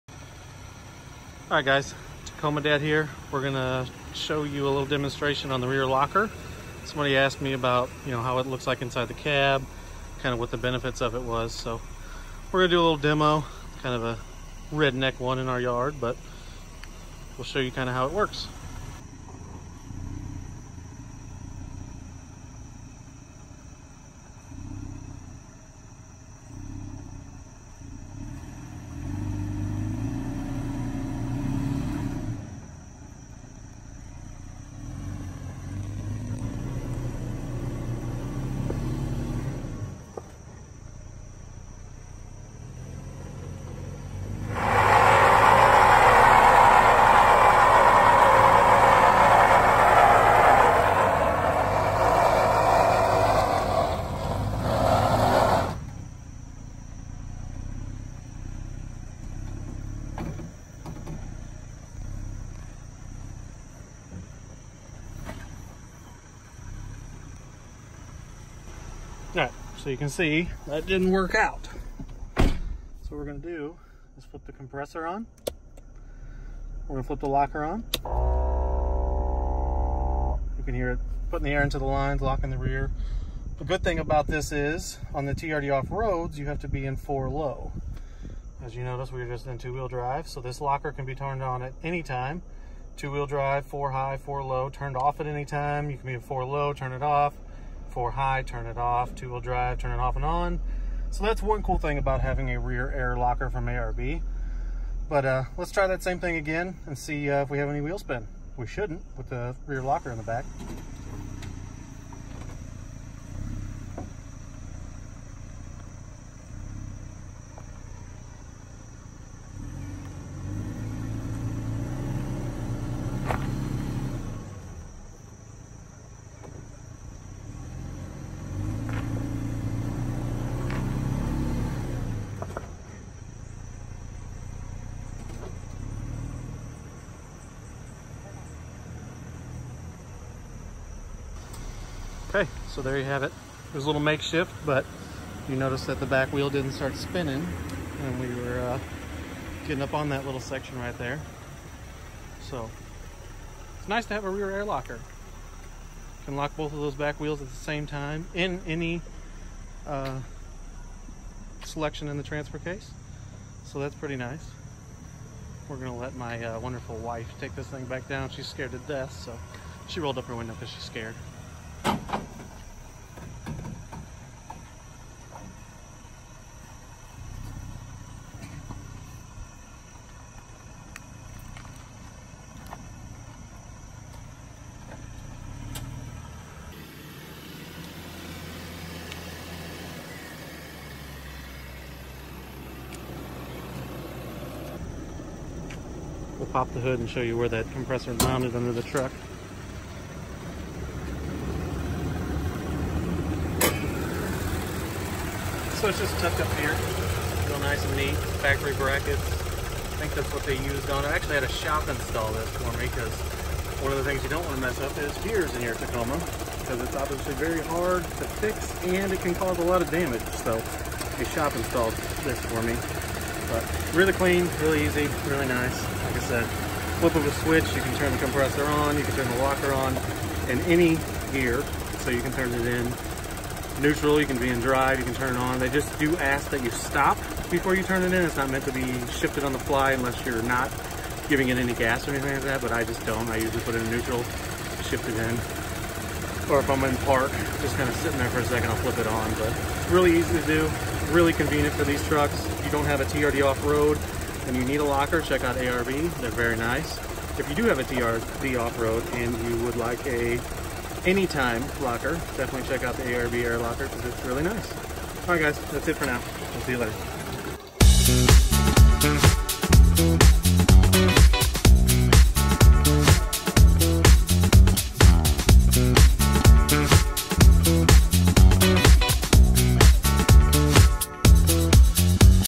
My FAVORITE addition to the truck 2018 Toyota Tacoma
2018 Toyota Tacoma | ARB air locker review

Demonstrating the effectiveness of an ARB rear air locker for the 3rd gen Tacoma. There is multiple reasons why your Tacoma needs a locker, in Offroad and everyday use. Works great in the snow, ice or anywhere both rear wheels needing traction comes into play. A strong advantage is it can be turned on in any selection on the transfer case, 2wd, 4hi and 4lo.

I highly recommend having a professional shop or certified mechanic to install the locker as there are many parts that can be damaged upon install or after installation. Parts and labor in my area were roughly 2k. Well worth every penny.

Check out battlebornoffroad.com to purchase yours, make sure to tell them:
Year of truck
Model/trim